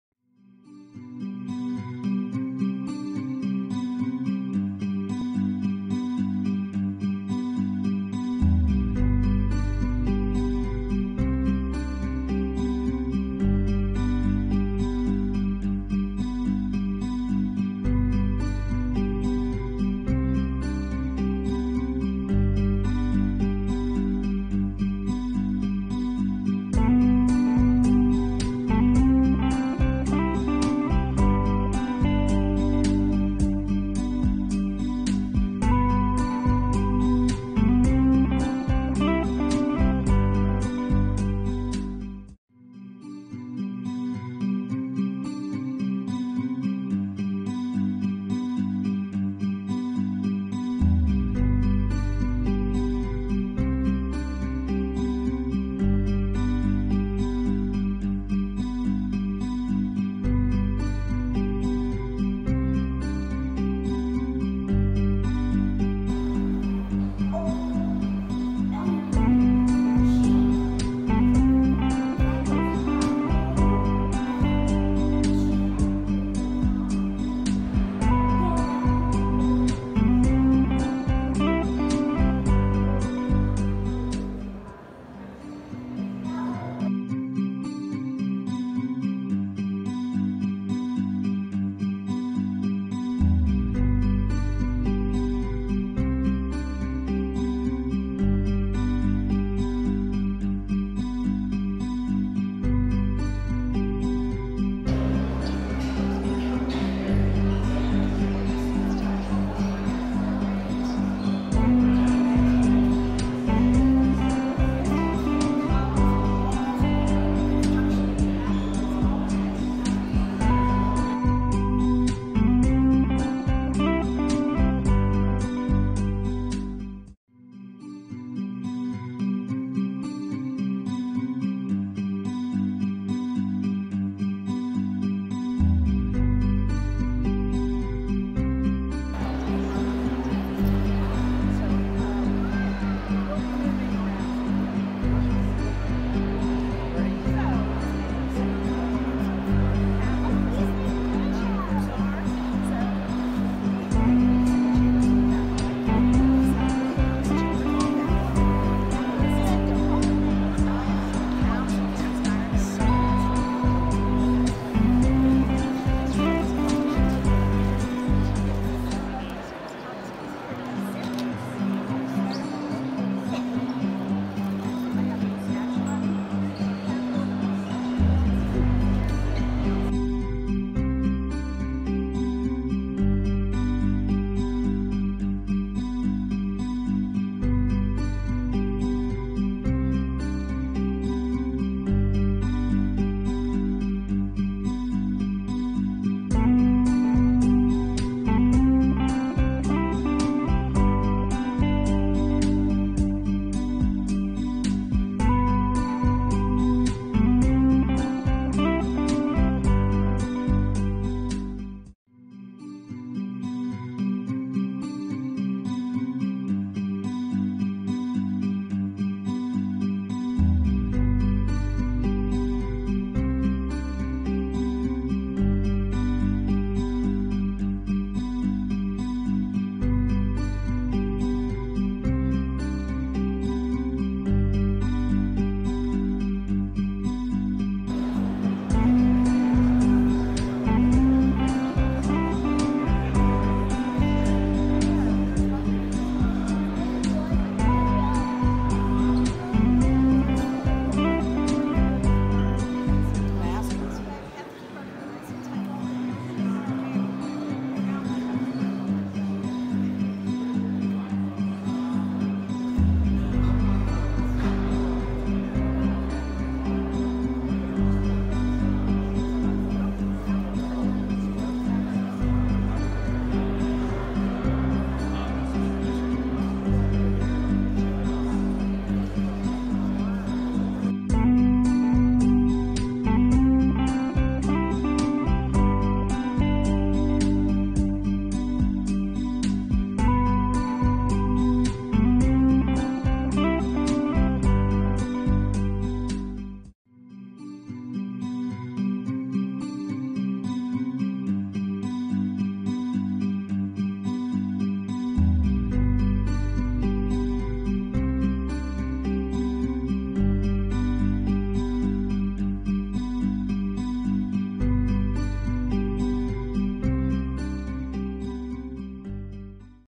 TNT Travels - Washington DC - Supreme Court & Capital Building
Today we got to tour the Supreme Court and Capital Building. We also sat in the gallery for a session of the Senate. Such beautiful buildings!